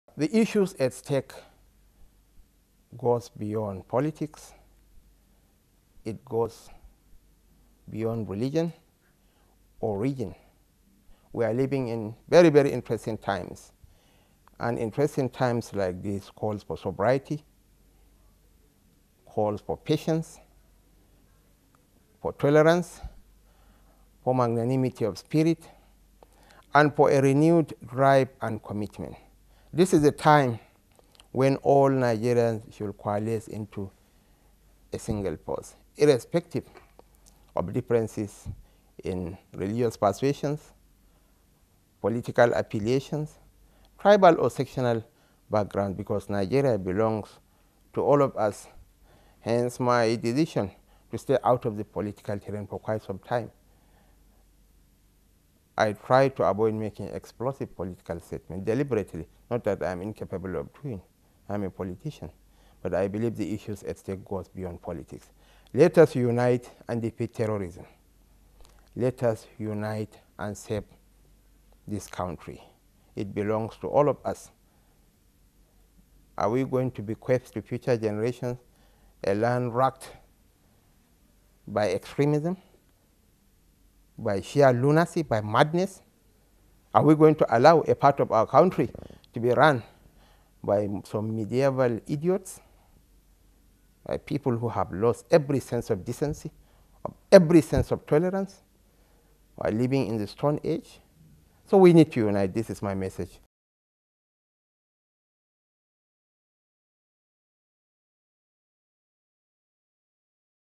Nigerians Must Unite In Fight Against 'Mediaeval Idiots' - Shettima
The Governor of Borno State Kashim Shettima, has reiterated the need for all Nigerians to unite in the fight against the 'mediaeval idiots', the Boko Haram sect, that has carried out series of attacks in the north-east region.